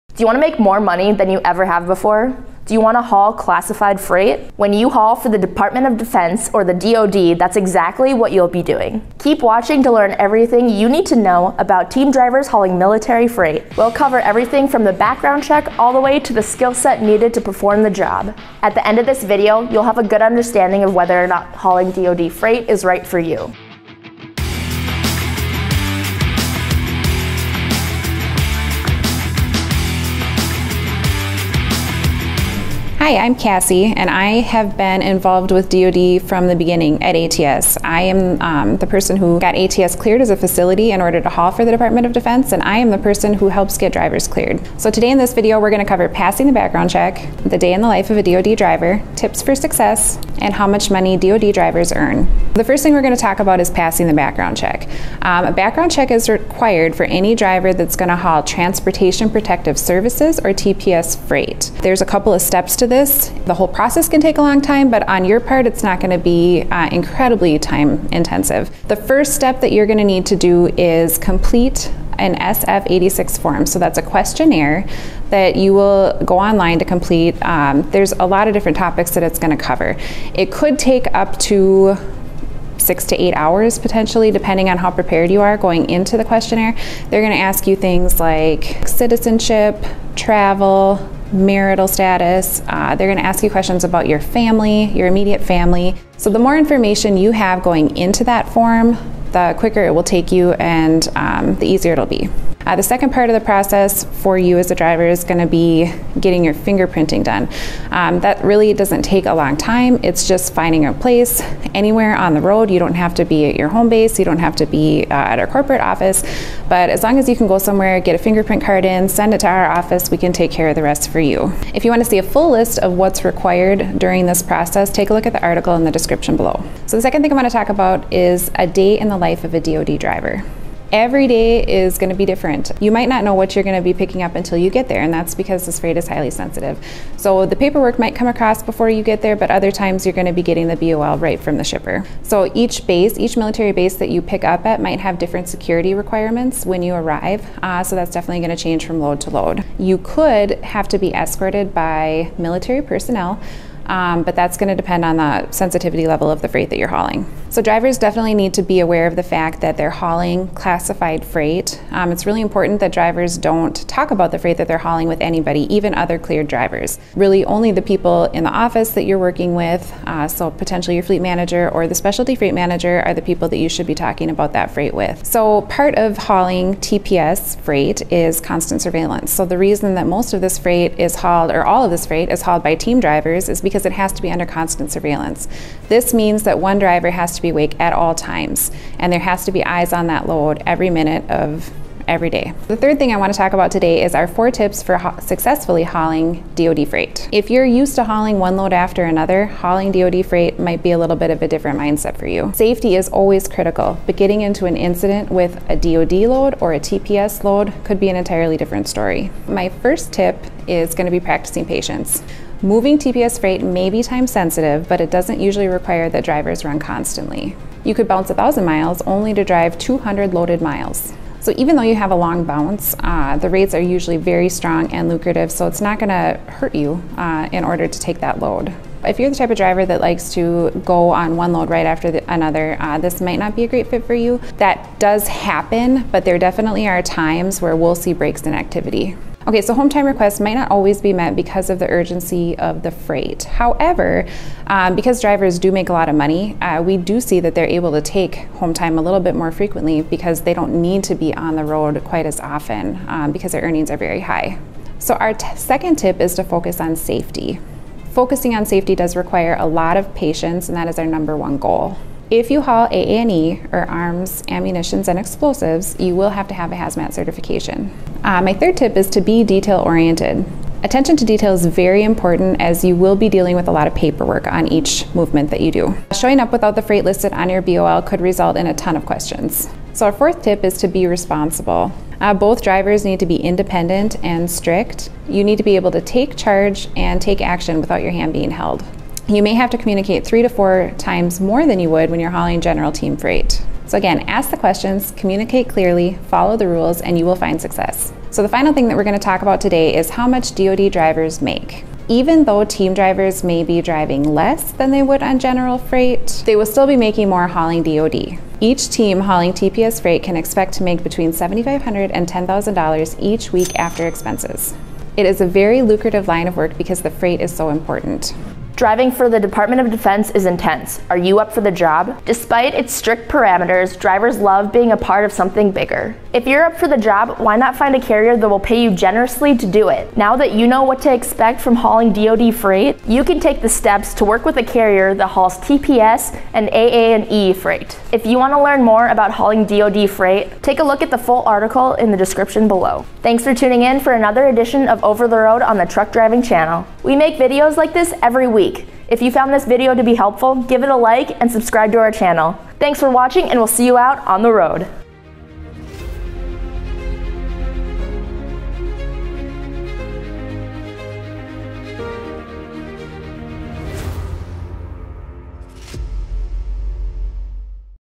DOD Military Freight: Do You Have What it Takes?
Do you want to haul classified freight? If you’re interested in hauling freight for the Department of Defense (DOD), this video will answer all of your questions.

Cassie will cover everything you need to know about DOD freight, from the background check to a day in the life of a DOD driver to how much money you can expect to make. She’ll also cover tips for success as a DOD driver.